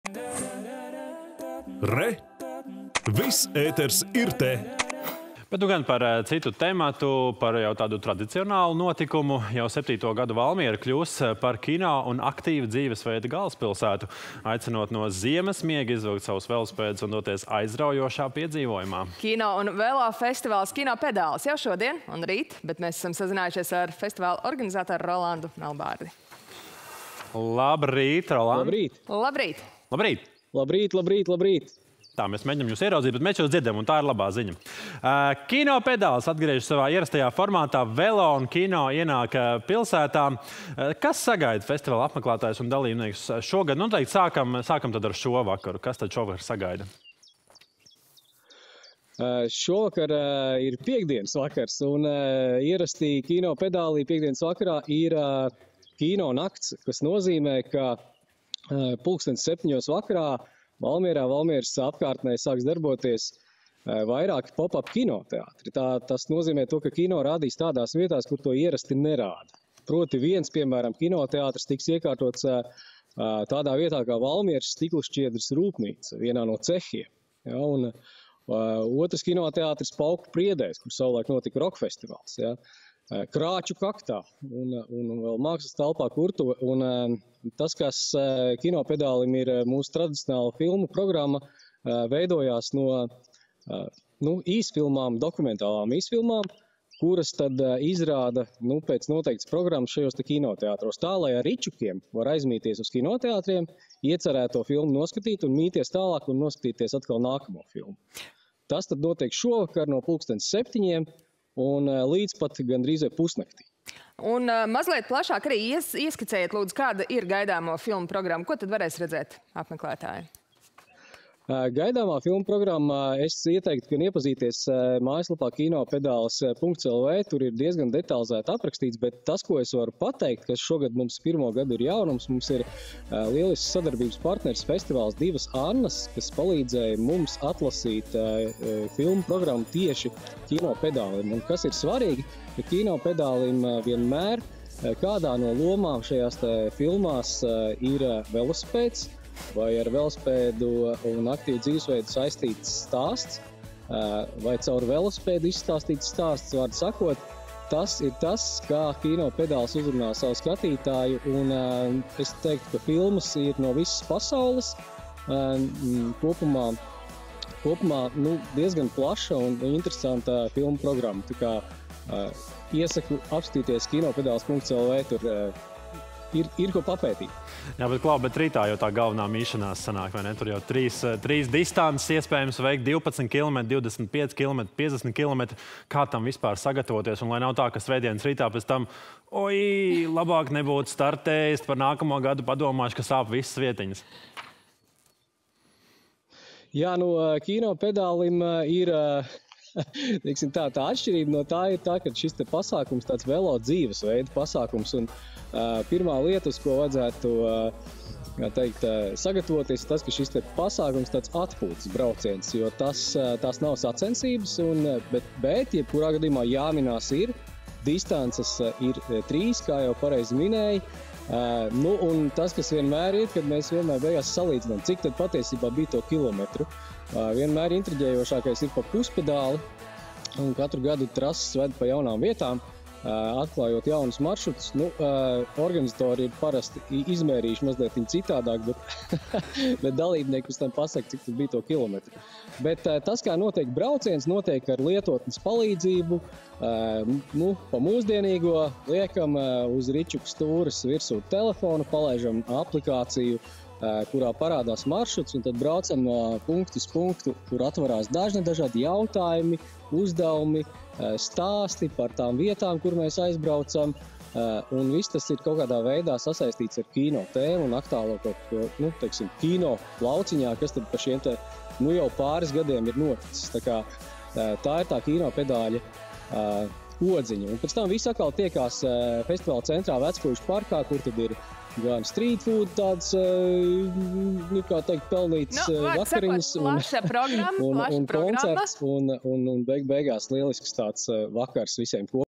Intervija ar kino un velo festivāla “Kino pedālis” organizatoru Rolandsu Melbārdi
Jau septīto gadu Valmiera kļūs par kino un aktīva dzīvesstila galvaspilsētu, aicinot no ziemas miega izvilkt savus velosipēdus un doties aizraujošā piedzīvojumā.
Kino un velo festivāls “KINO PEDĀLIS” jau šodien un rīt. Esam sazinājušies ar festivāla organizatoru Rolandu Melbārdi.